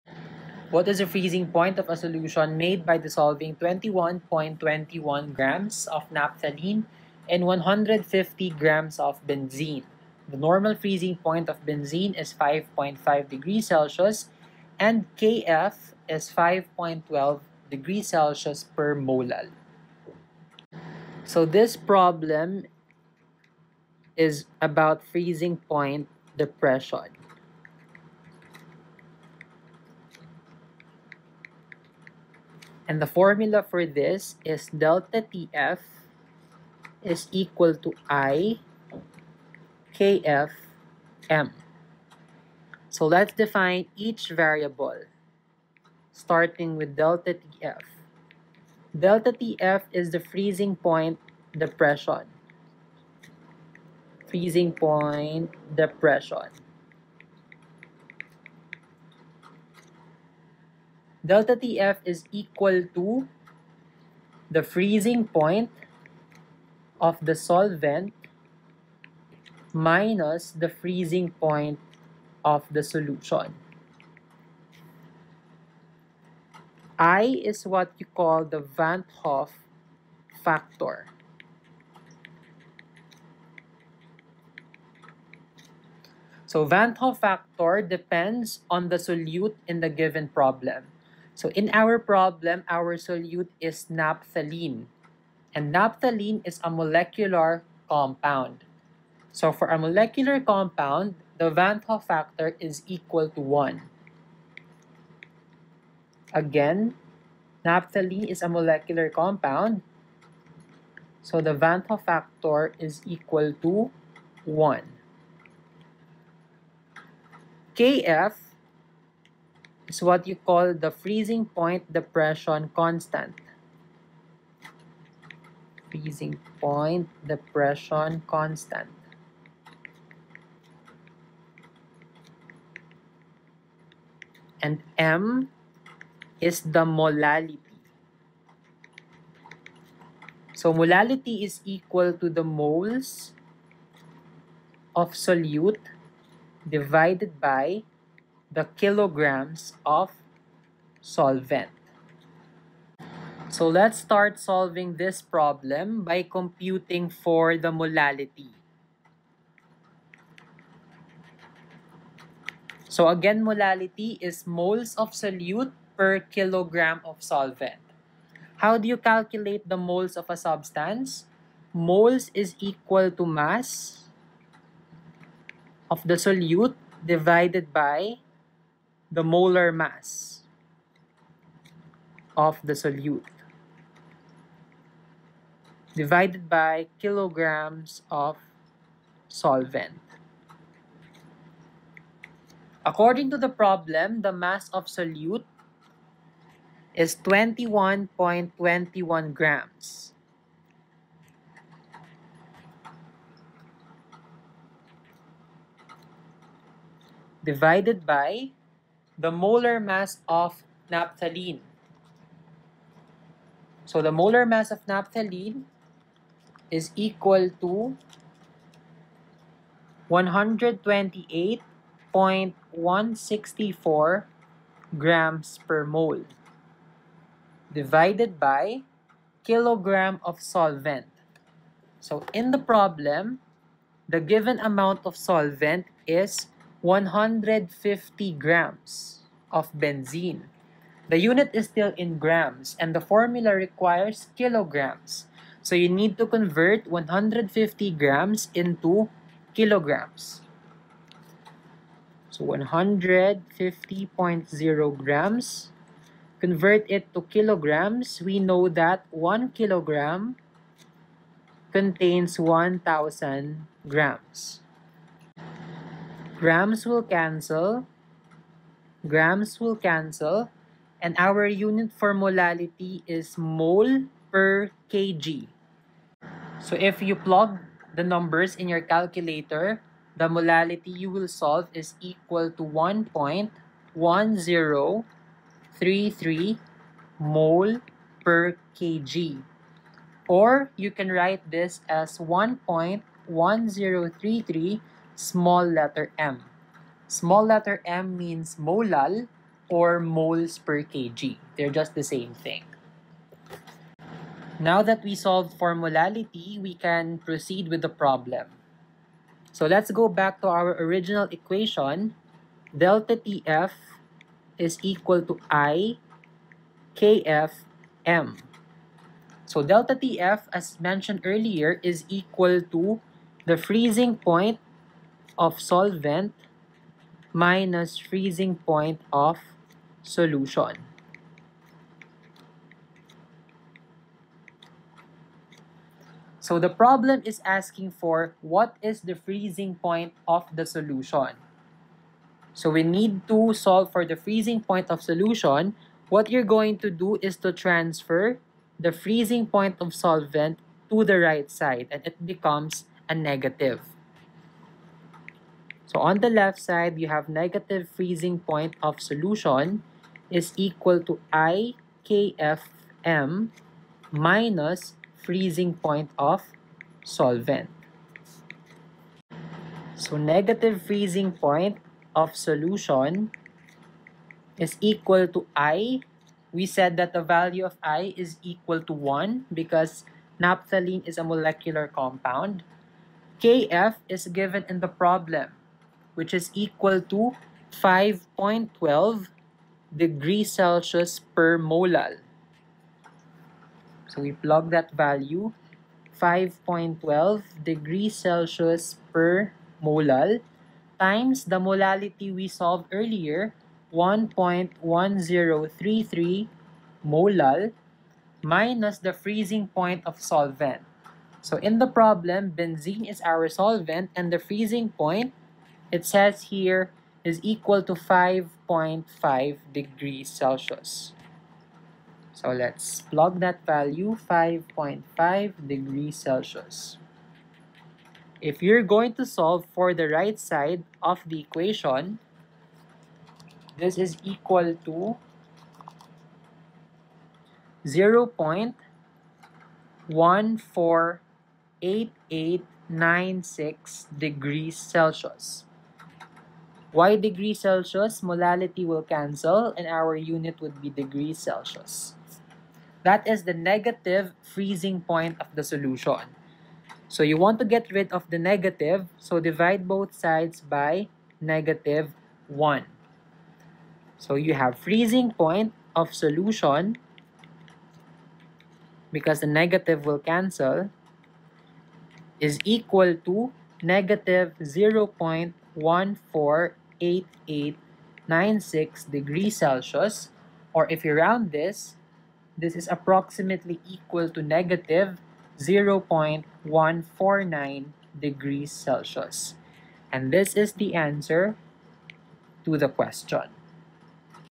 What is the freezing point of a solution made by dissolving 21.21g of naphthalene (C10H8) in 150.0 g
What is the freezing point of a solution made by dissolving 21.21 g of naphthalene (C10H8) in 150.0 g benzene (C6H6?)? The normal freezing point of benzene is 5.5°C and Kf is 5.12°C/m.

How do you know which is the solute and the solvent?
The solvent is the substance present in greater amount. Therefore, benzene (150.0g) is the solvent and naphthalene (21.21g) is the solute.